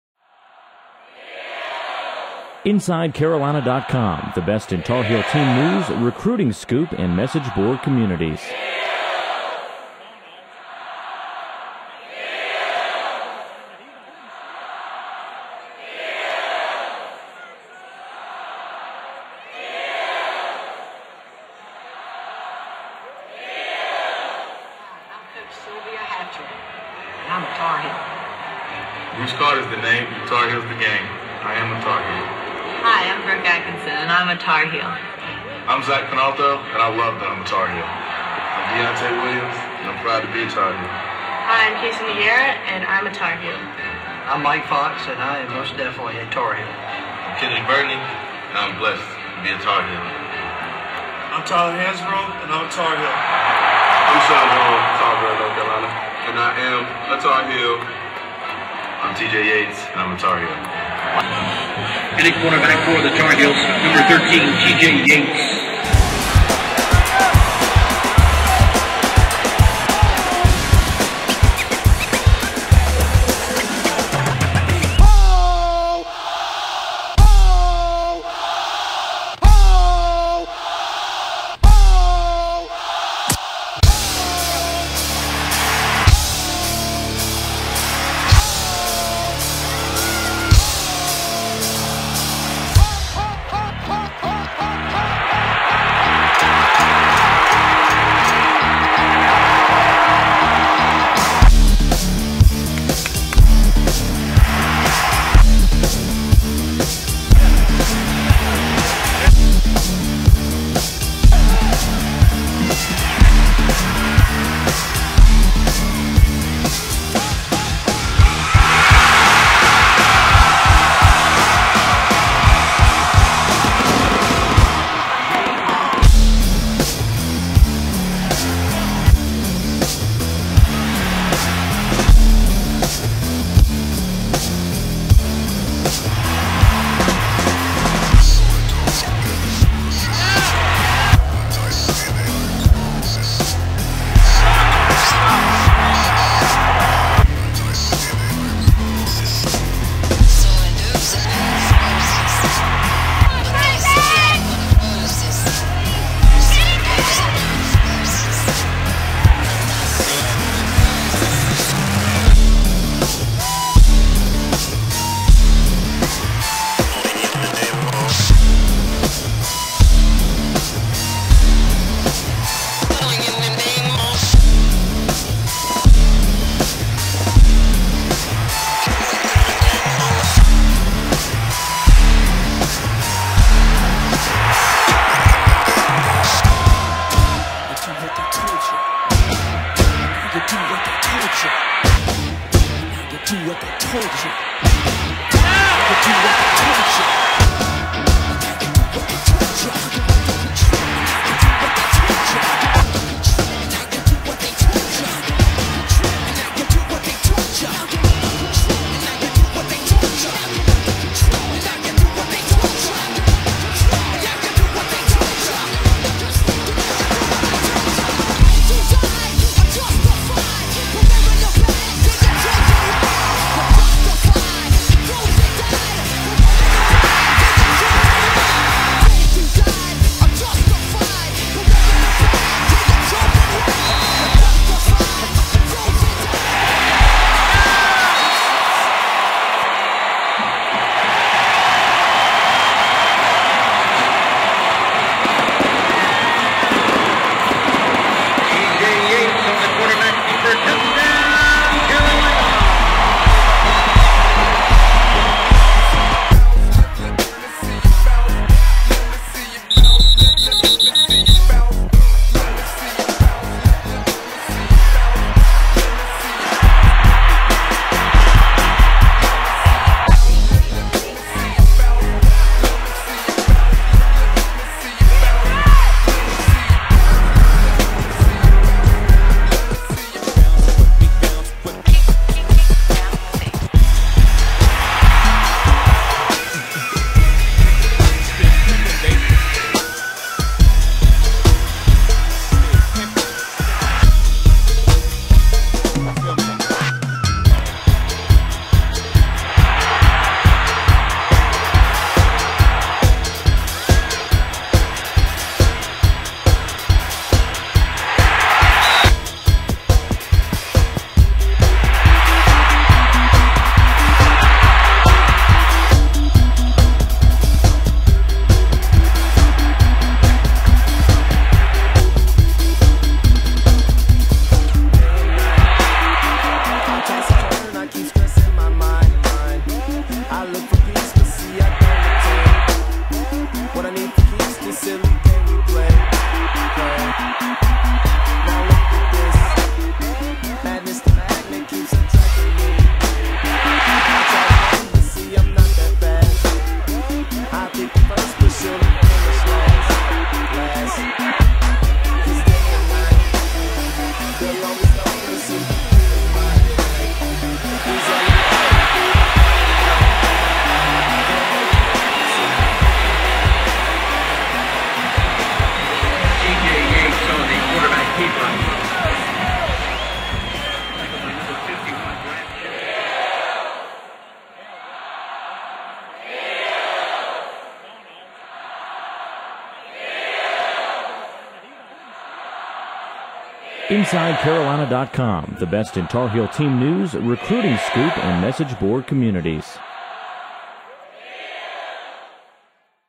InsideCarolina.com presents "T.J. Yates: Tar Heel" 2010
InsideCarolina.com presents "T.J. Yates" 2010
Highlights from the North Carolina Tar Heels
2010 Football Season with the Focus of this video
on Senior QB T.J. Yates. With video from all home games
and the Duke game
video: Vinny Savage